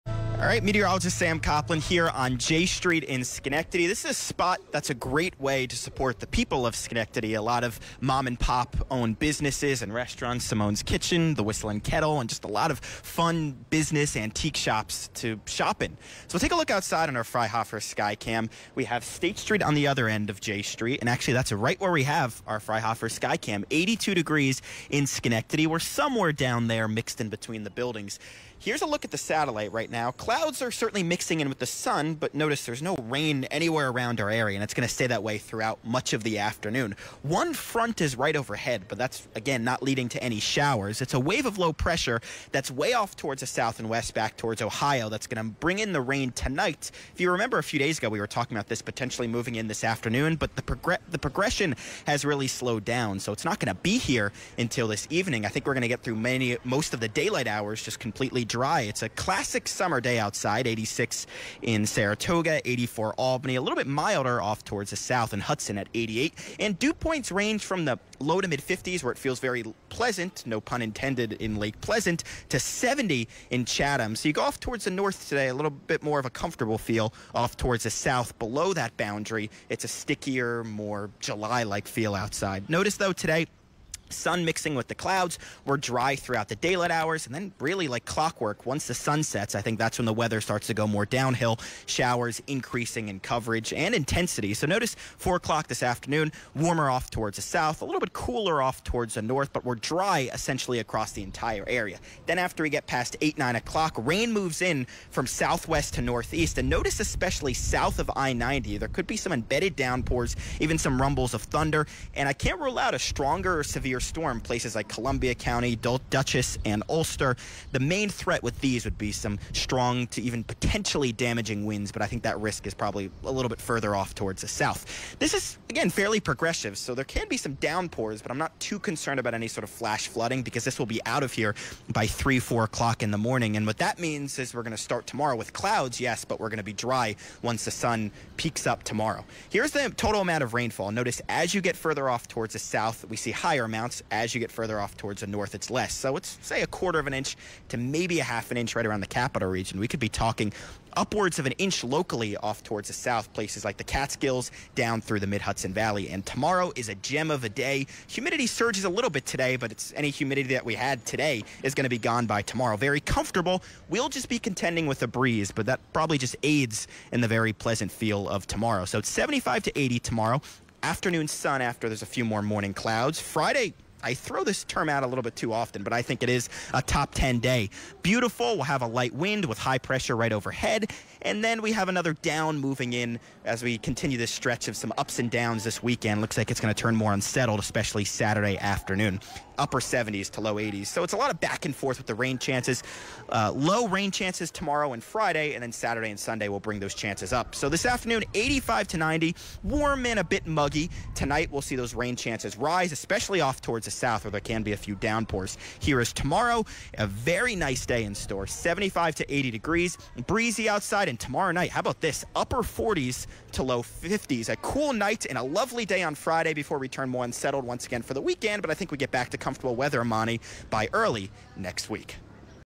Sam Coplin 6/26/24 Noon Weather Forecast
A warm but mainly dry day today before we bring those shower/storm chances back into the area for tonight. A few storms can be strong.